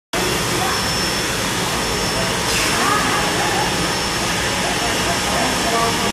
CE mark overhead crane vacuum lifting device tube lifter for lifting carton and cardboard box
customize pneuamtic manipulator